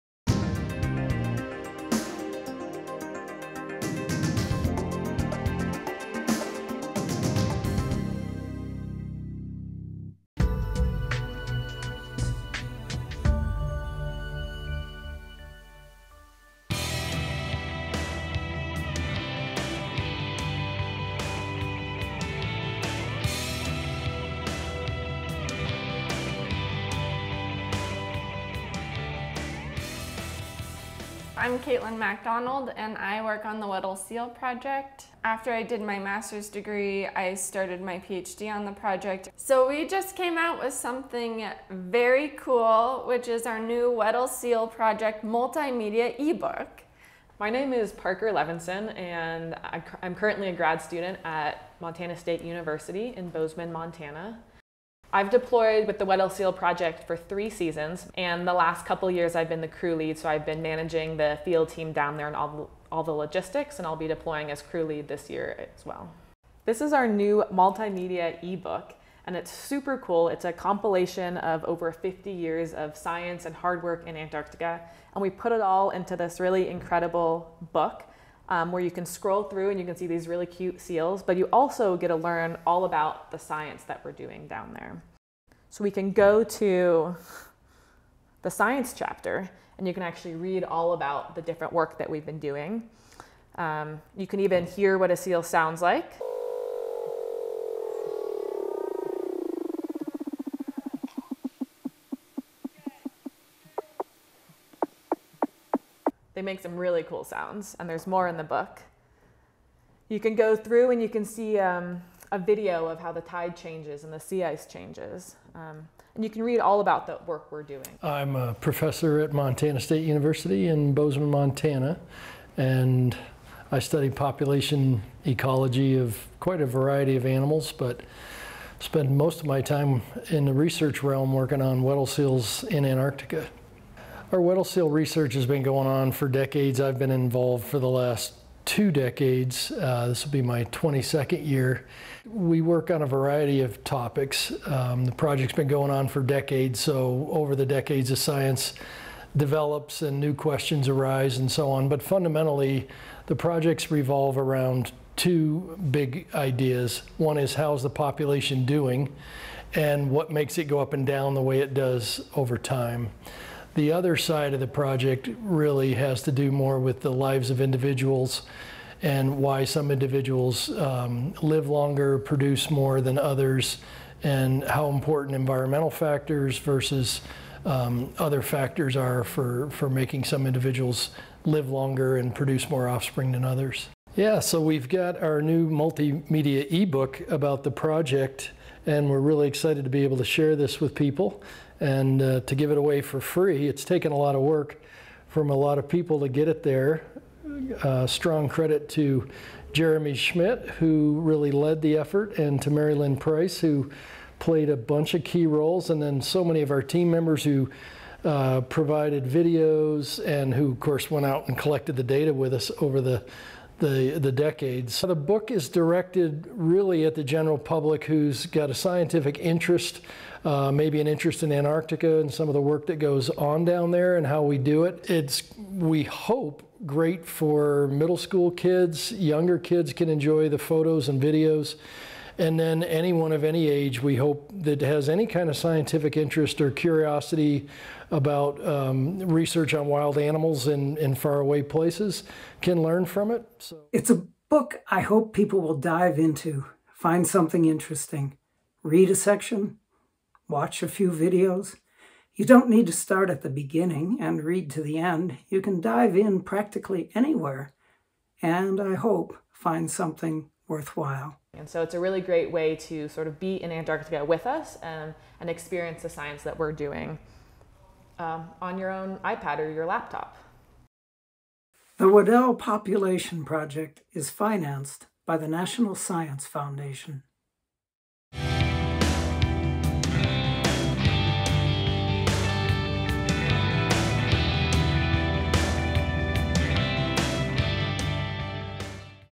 Our New Multimedia eBook On Weddell Seals!
Weddell Seal Science has published a free multimedia ebook titled, "Weddell Seals; Science, Life History, and Population Dynamics"! This brief video includes interviews about the new ebook with Weddell Seal Population Project lead scientist Prof. Jay Rotella, PhD student on the project Kaitlin Macdonald, Masters student/field crew leader Parker Levinson, and ebook creator Jeremy Schmidt. The new ebook is available for FREE at Apple Books, and includes numerous cool photos, videos, graphics and audio accompanying the engagingly written chapters.

Writer and creator Jeremy Schmidt has produced a beautiful and informative science-oriented ebook that can be enjoyed by all ages, from young people to those interested in lifetime learning. Jeremy is a writer and multimedia specialist with the Weddell Seal Population Project,. The project is funded by the National Science Foundation, through a grant to Montana State University. Video production by Mary Lynn Price. The views and comments in this ebook do not represent those of Montana State University, and should not be construed as such.